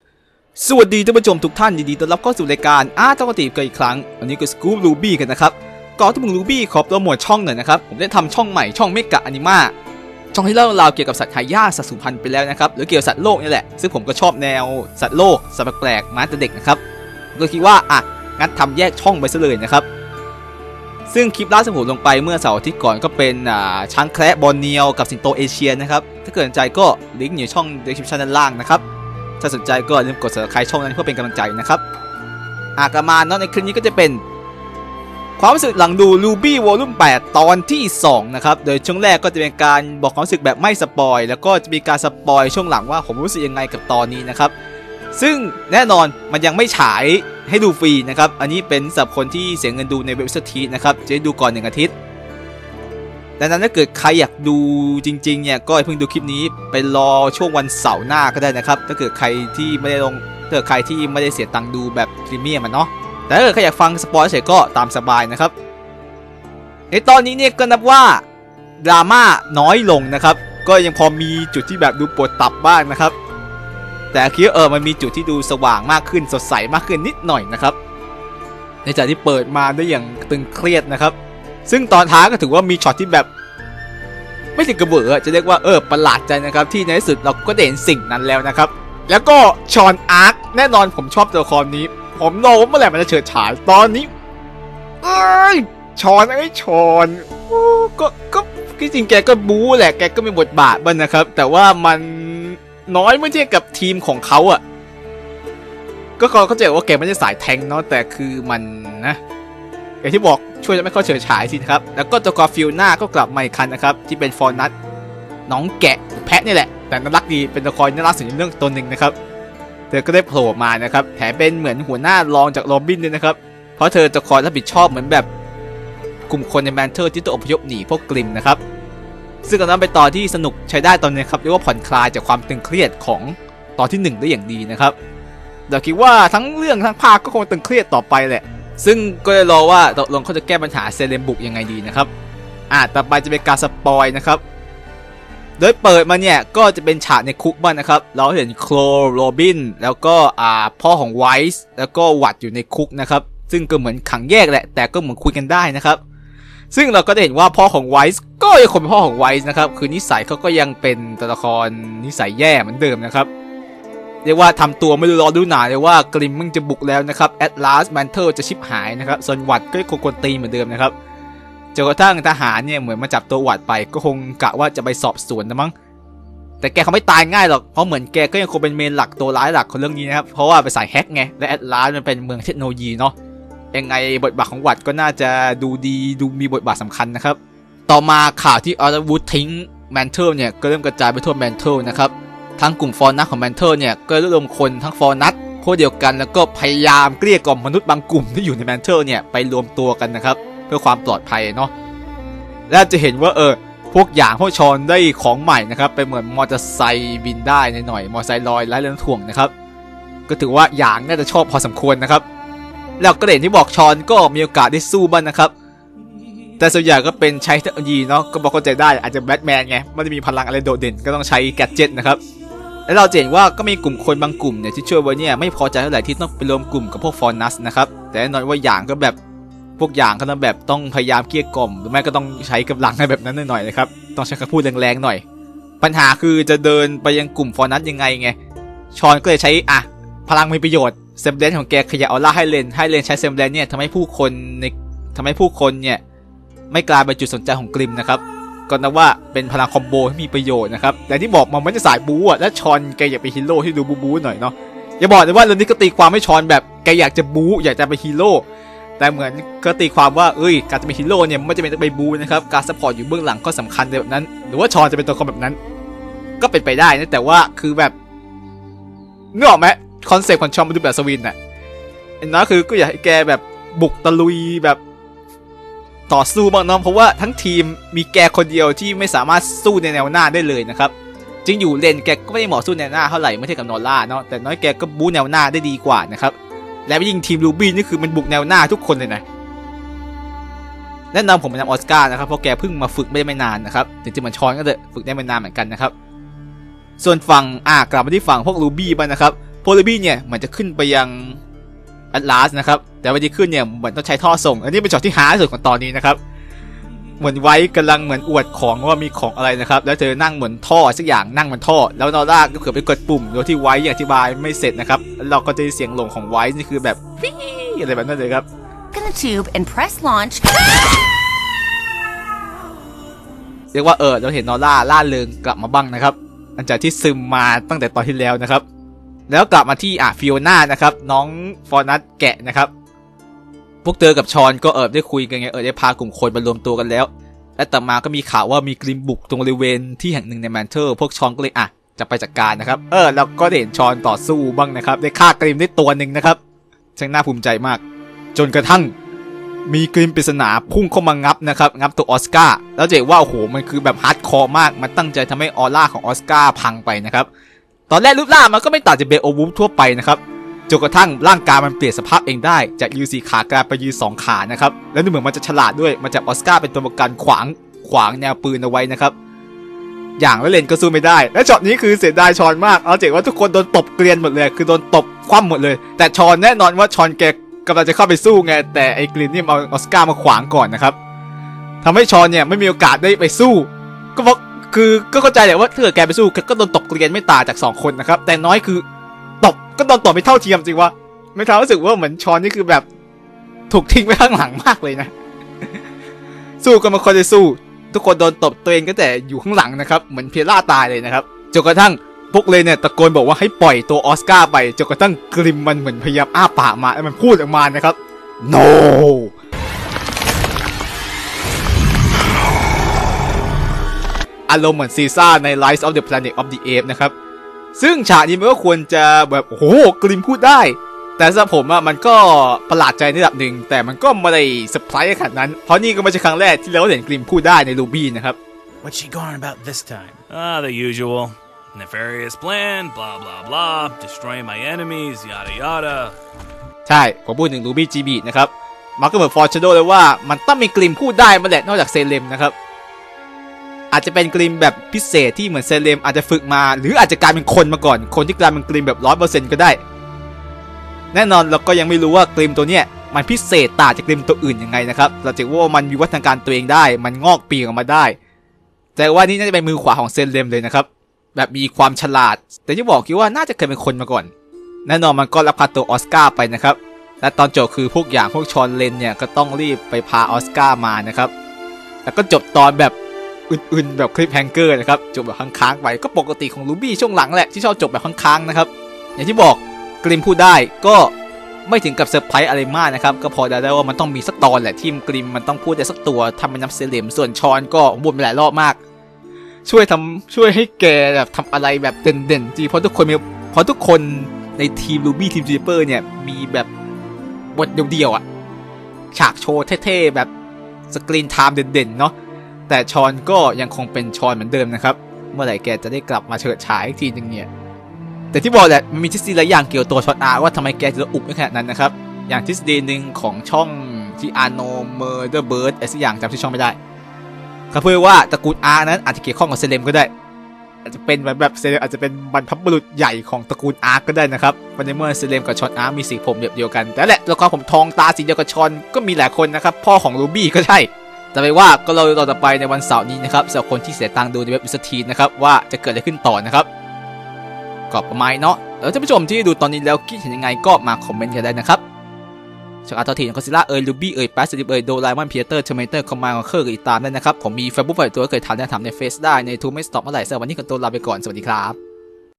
ความรู้สึกหลังดู RWBY Volum 8 ตอนที่ 2 : สงสาร Weiss
ในคลิปนี้ผมจะมารีวิว RWBY ภาค 8 ตอนที่ 2 กันนะครับ

Link Mecha Anima

ท่านสามารถสนับสนุนช่องผมได้จากช่องทางด้านล่างนะครับ ขอบคุณที่สนับสนุนและติดตามนะครับ
พร้อมเพลย์: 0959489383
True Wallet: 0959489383